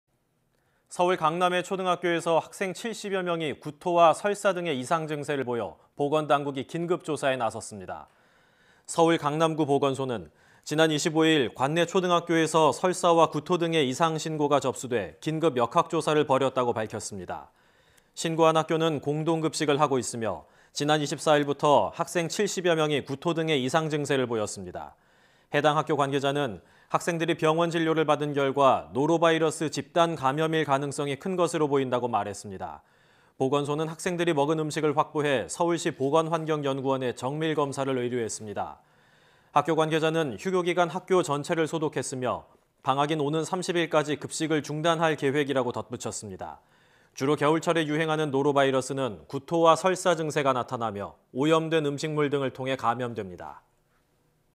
[단독] 서울 강남 초등학생 70여 명 집단 설사..."노로바이러스 의심" / YTN 사이언스
서울 강남의 초등학교에서 학생 70여 명이 구토와 설사 등의 이상 증세를 보여 보건 당국이 긴급 조사에 나섰습니다.

서울 강남구보건소는 지난 25일 관내 초등학교에서 설사와 구토 등의 이상 신고가 접수돼 긴급 역학조사를 벌였다고 밝혔습니다.

신고한 학교는 공동 급식을 하고 있으며, 지난 24일부터 학생 70여 명이 구토 등의 이상 증세를 보였습니다.

해당 학교 관계자는 학생들이 병원 진료를 받은 결과, 노로바이러스 집단 감염일 가능성이 큰 것으로 보인다고 말했습니다.

하지만 강남구보건소는 지난 21일부터 23일까지의 급식 메뉴 가운데 상하기 쉬운 음식은 없었다며, 독감일 가능성도 배제하지 않고 있습니다.

보건소는 학생들이 먹은 음식물을 확보해 서울시 보건환경연구원에 정밀검사를 의뢰했습니다.

학교 관계자는 오늘까지 구토 등의 이상 증세를 보인 학생은 39명이며, 추가 전파를 막기 위해 어제와 오늘 이틀 동안 휴교를 했다고 밝혔습니다.

또, 휴교 기간 학교 전체를 소독했으며 방학인 오는 30일까지 급식을 중단할 계획이라고 덧붙였습니다.

주로 겨울철에 유행하는 노로바이러스는 구토와 설사 증세가 나타나며 오염된 음식물 등을 통해 감염됩니다.

지난달 이후 발생한 노로바이러스 감염 사례 10건 중 8건 이상은 어린이집과 유치원, 초·중·고등학교에서 집중적으로 발생했습니다.